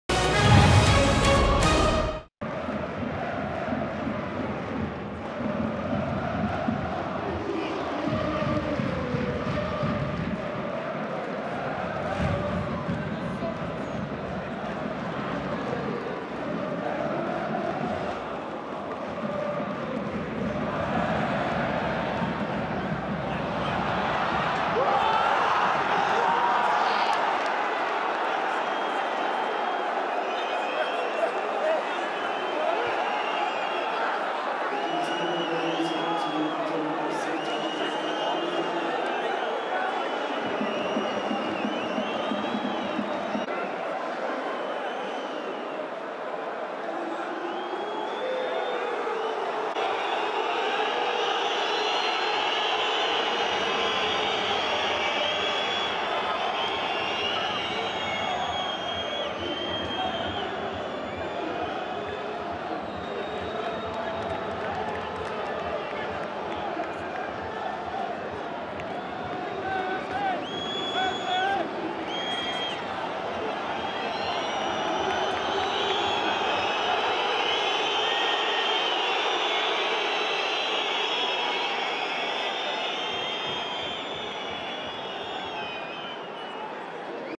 Goal Kalifa COULIBALY (28') / RC Strasbourg Alsace - FC Nantes (2-1) (RCSA-FCN) / 2019-20
RC Strasbourg Alsace - FC Nantes ( 2-1) Kalifa COULIBALY (28') goal. RC Strasbourg Alsace vs FC Nantes Goals in video

Ligue 1 Conforama - Season 2019/2020 - Week 6 Stade de la Meinau - Friday 20 September 2019

Goals : Kalifa COULIBALY (28' - FC Nantes) / Dimitri LIENARD (66' - RC Strasbourg Alsace) / Ludovic AJORQUE (89' pen - RC Strasbourg Alsace)